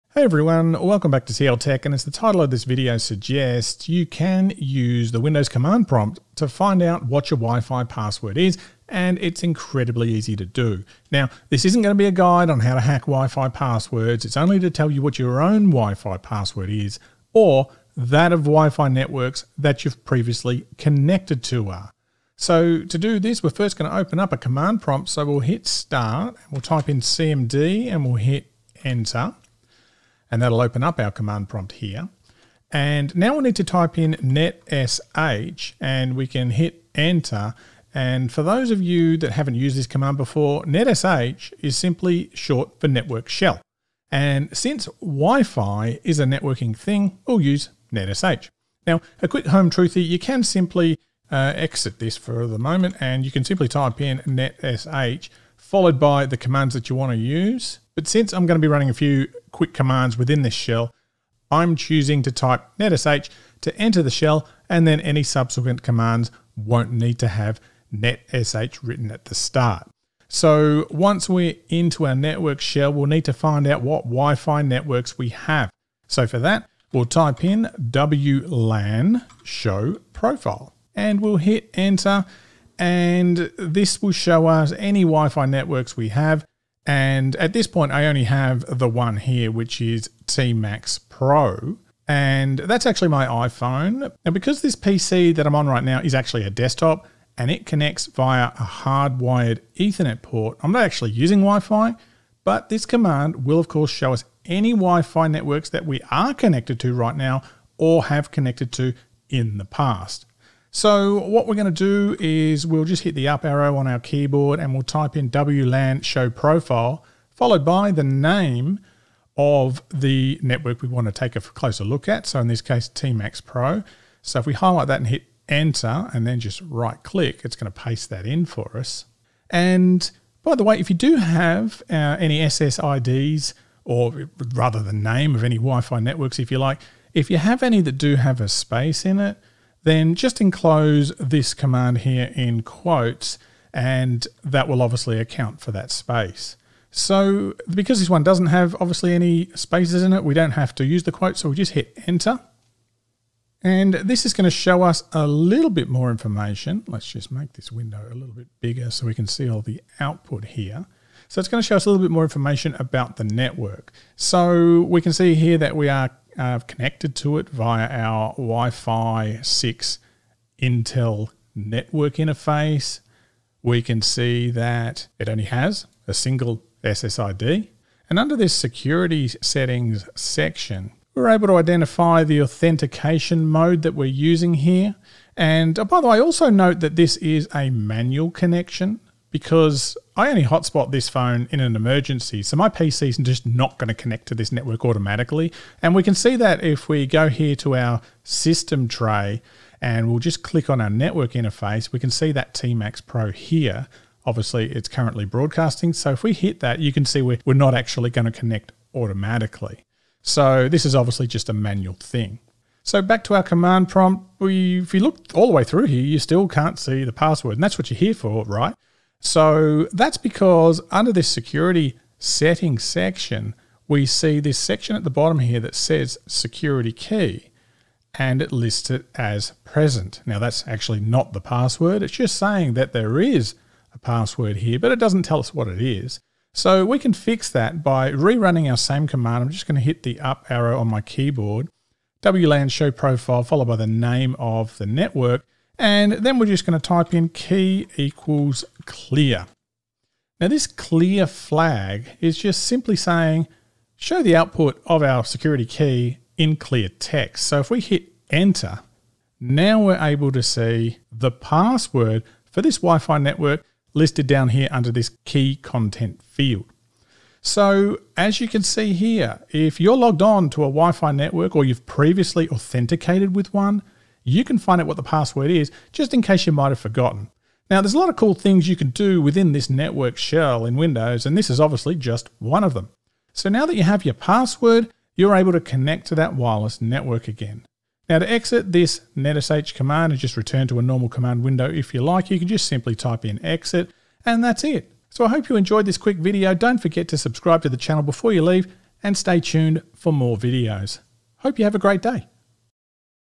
You can find your WIFI Passwords from the Windows Command Prompt!
Forget your WIFI password? Yes it's true if you've ever connected to a WIFI network before, you can use the command prompt to find out what the password is. In this video we'll show you how.

4:06 is where you'll learn the actual command :)